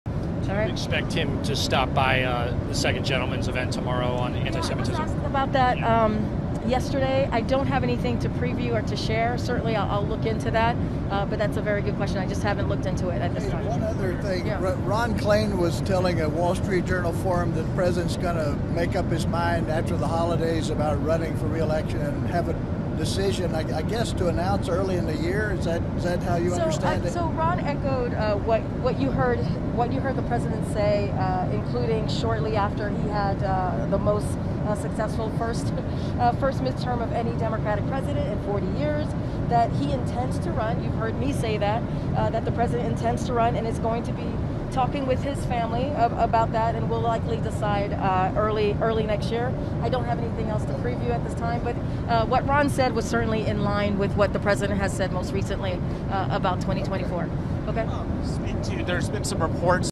KJP Asked Point Blank: When Will Biden Announce 2024 Intentions?
Karine Jean-Pierre is asked about President Biden's 2024 plans.